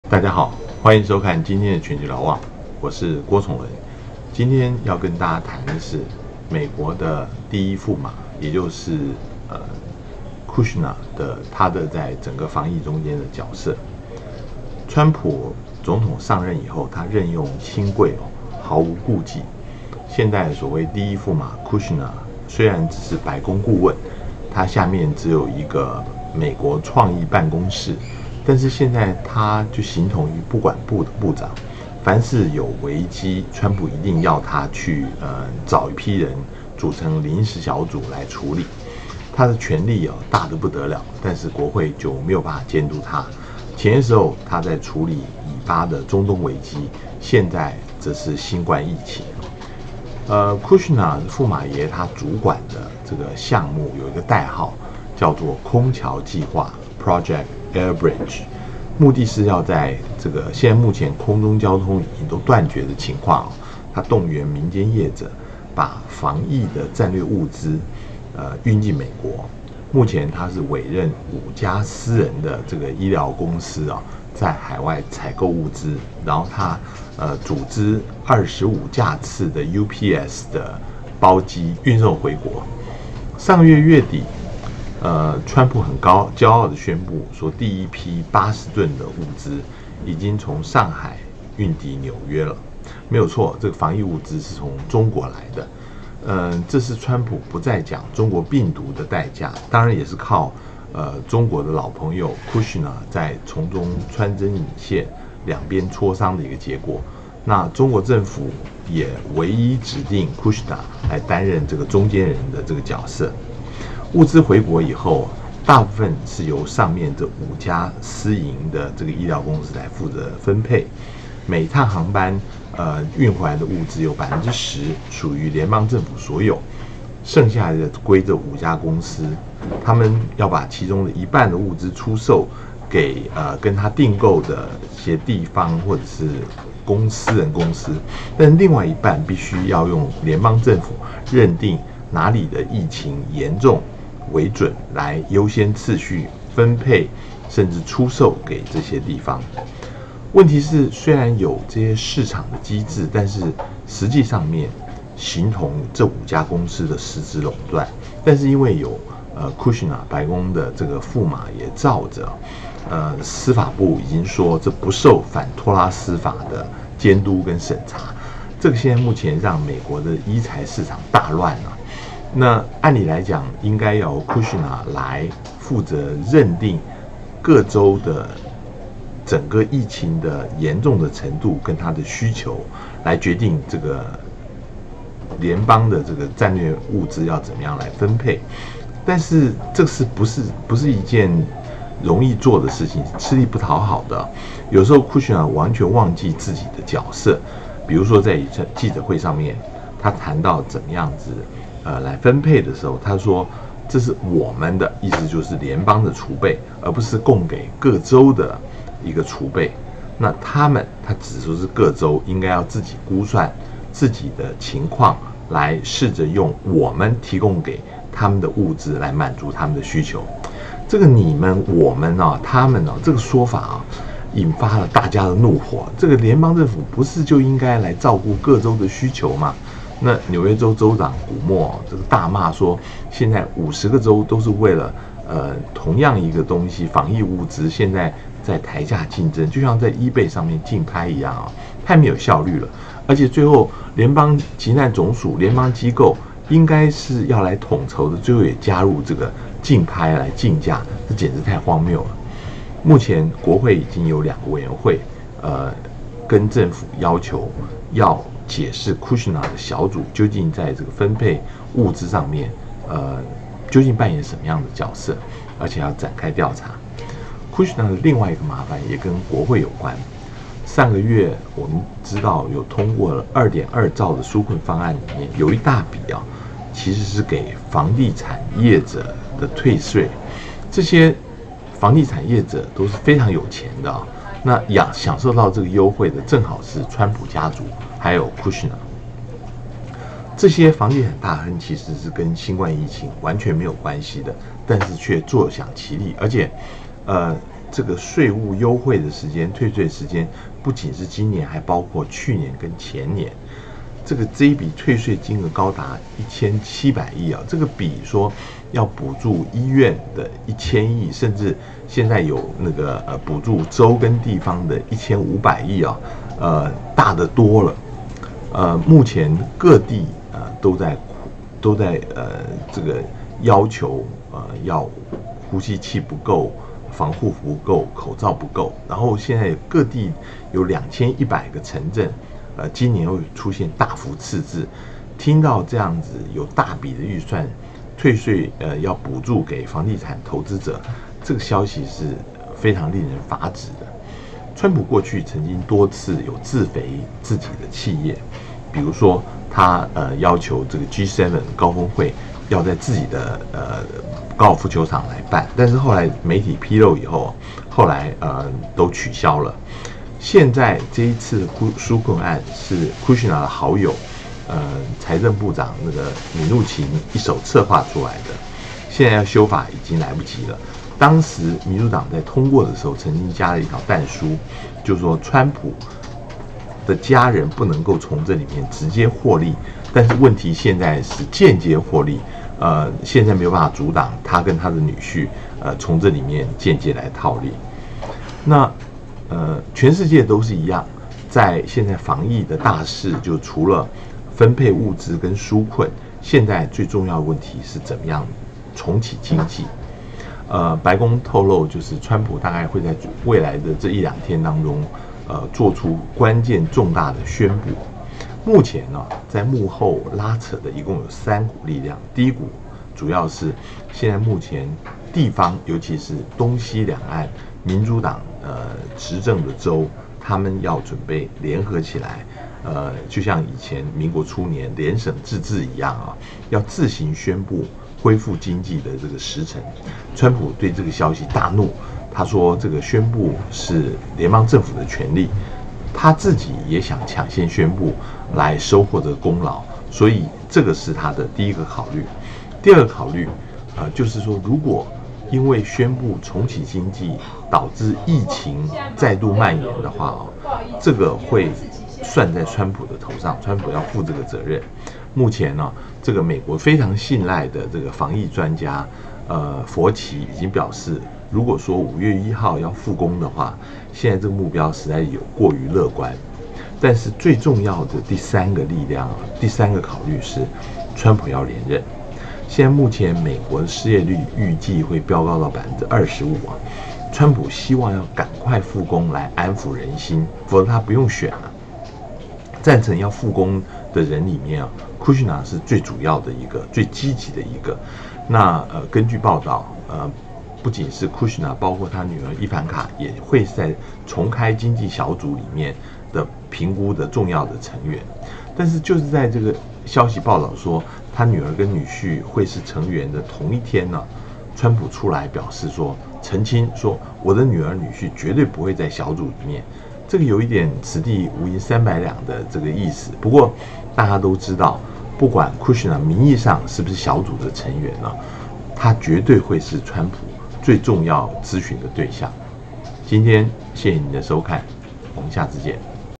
0417第一駙馬庫許納 包辦防疫與紓困？
川普任用親貴，毫不顧忌，第一駙馬庫許納身為白宮顧問，時常肩負重要任務，前一陣子是以巴中東方案，現在則是新冠疫情，代號「空橋計畫」，負責調度防疫物資，當中國大陸運來的防疫物資，到了美國本土之後，到底該如何分配？而在2.2兆紓困方案中，有一大筆是給房地產投資者的退稅，享受到這個優惠的正好是川普家族，以及他的女婿庫許納。川普過去曾經多次自肥自己的企業，民主黨雖然加上但書，不准川普家人從紓困案中直接獲利，但是卻阻止不了他與他的女婿間接套利。全球瞭望由聯合報副總編輯郭崇倫，來談川普女婿庫許納，在防疫物資與紓困方案中所扮演的角色。